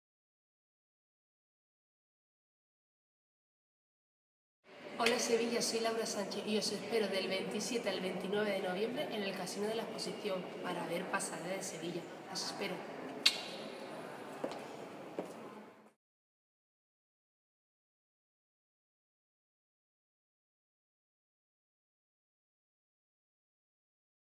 Laura Sánchez te da la bienvenida a Pasarela Sevilla
Del 27 al 27 de Noviembre, Pasarela Sevilla, donde camina la modadesevilla.